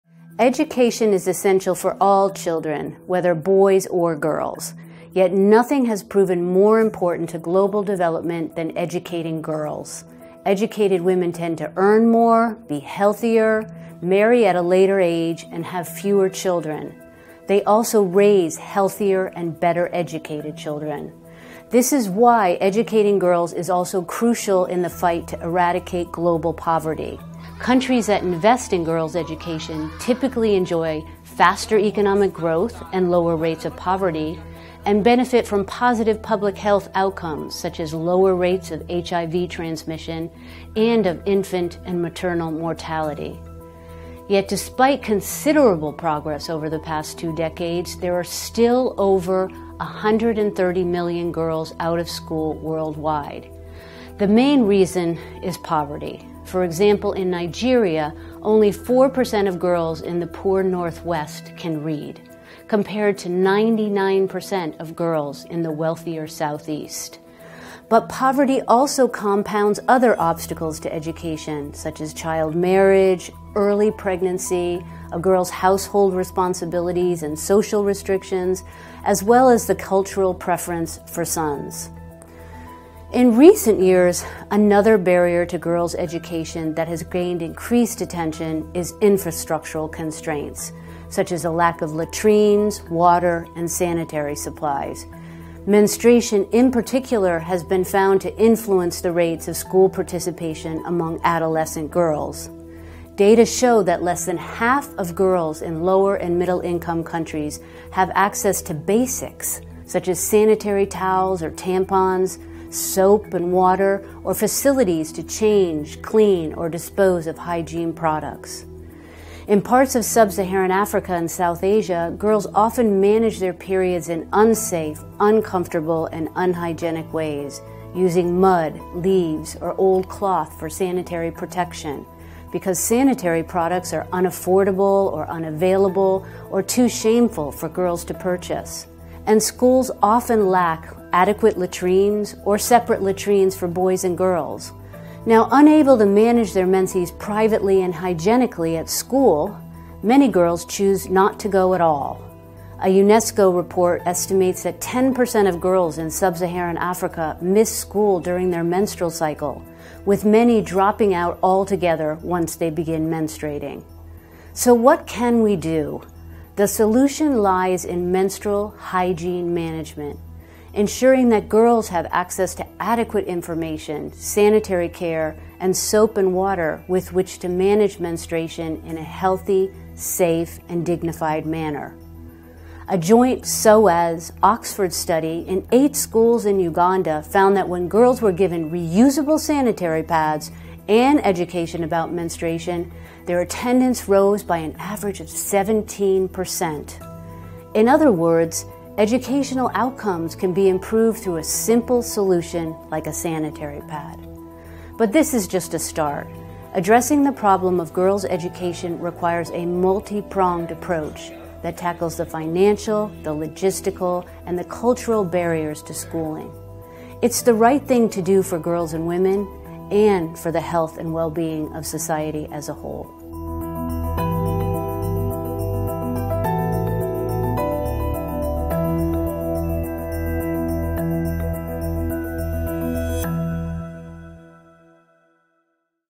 The Easy Solution to Help Keep Girls in School
An African proverb says, “If we educate a boy, we educate one person. If we educate a girl, we educate a family – and a whole nation.” So why are girls' rates of school attendance and literacy still so low in many countries?

For more PS, join the conversation on...